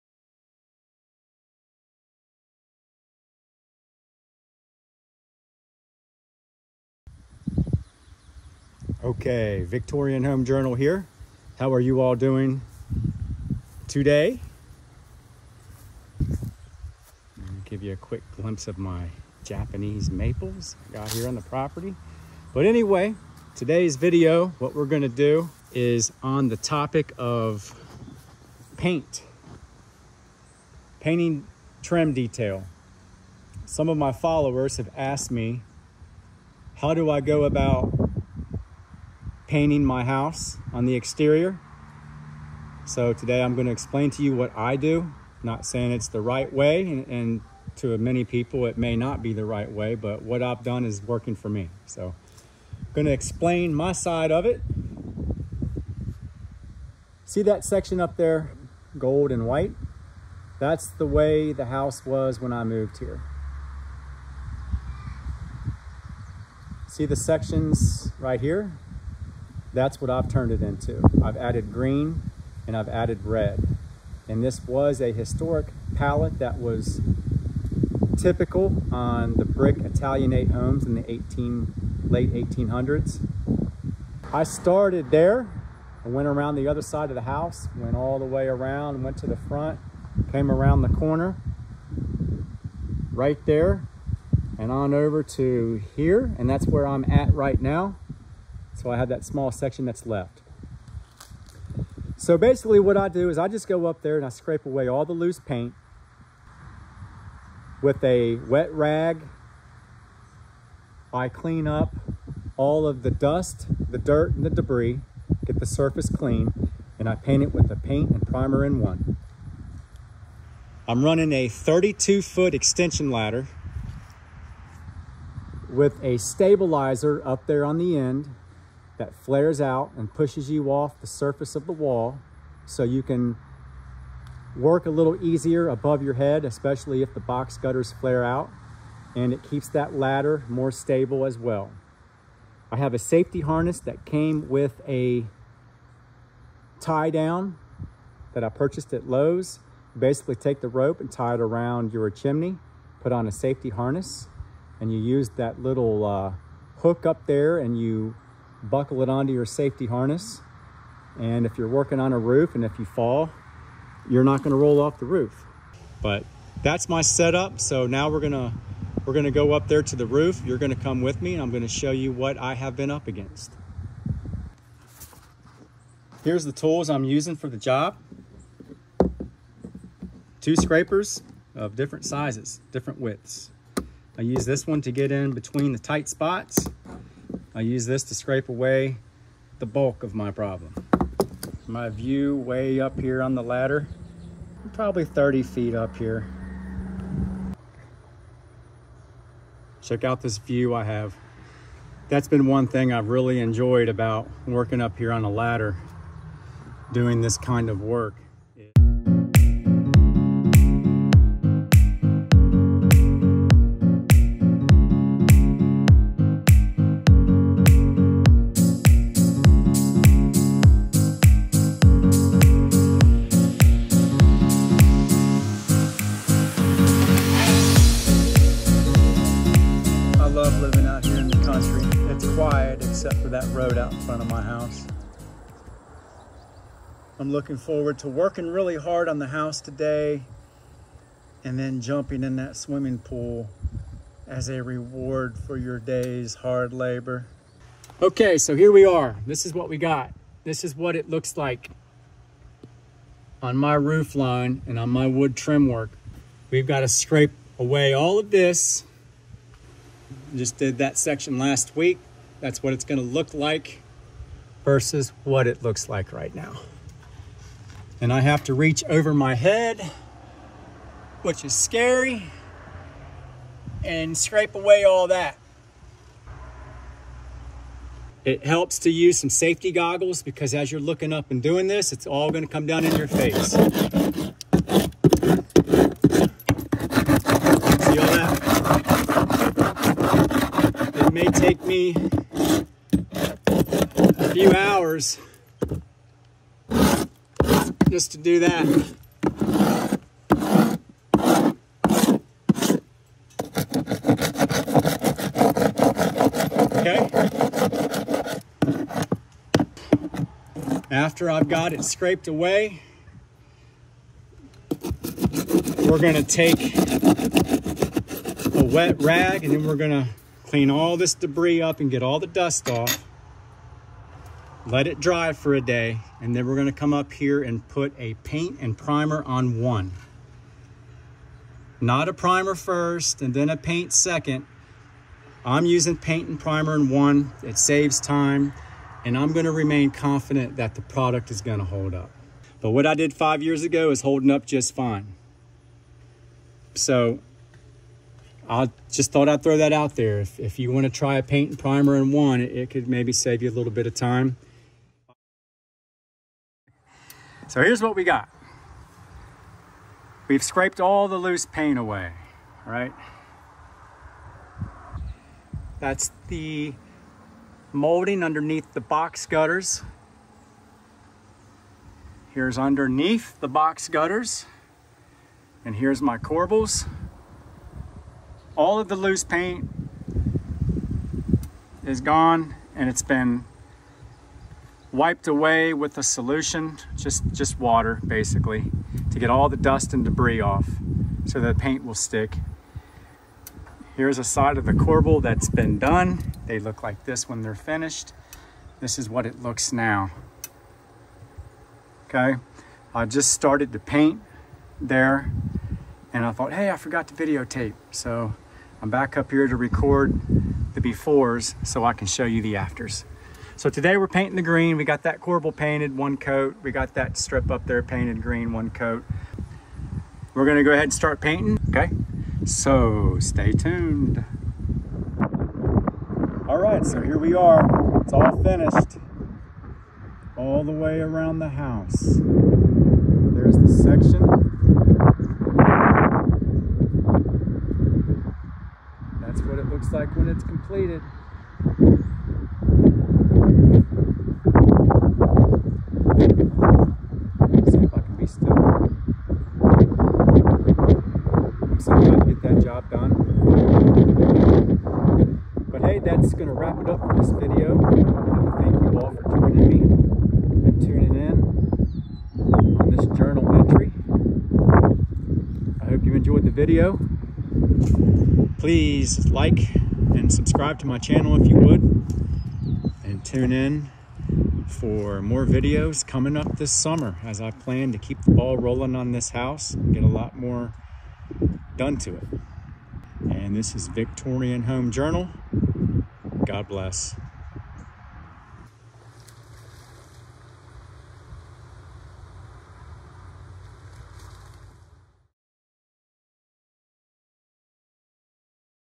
Journal Entry#8 Painting Exterior Trim
In this episode we scrape and paint exterior victorian trim work.